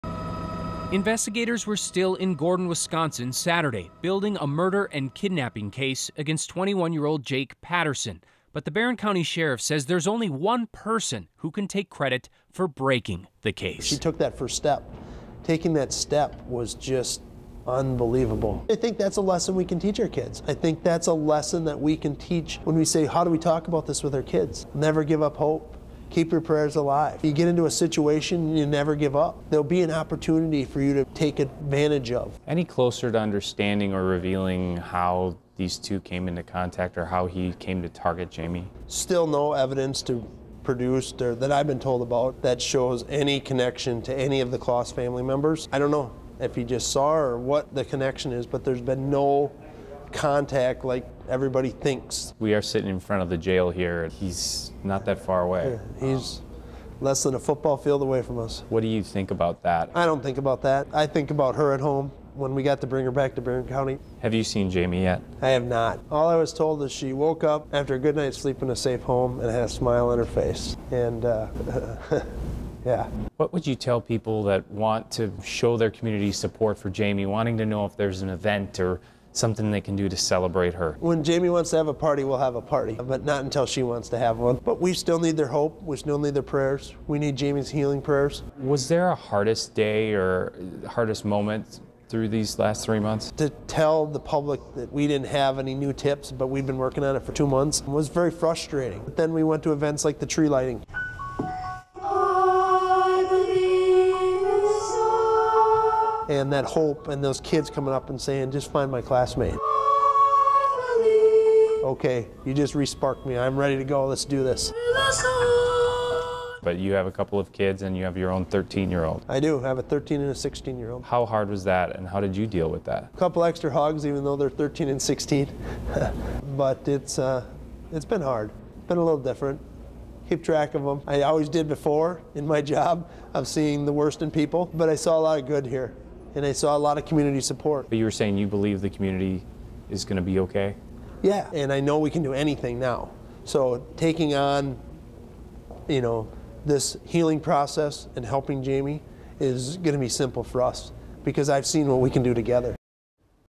Q&A with Barron Co. Sheriff Fitzgerald on Jayme Closs and her return
Investigators were still in Gordon, Wisconsin on Saturday, building a murder and kidnapping case against 21 year old Jake Patterson. But Barron County Sheriff, Chris Fitzgerald, says there's only one person who can take credit for breaking the case: 13 year old Jayme Closs.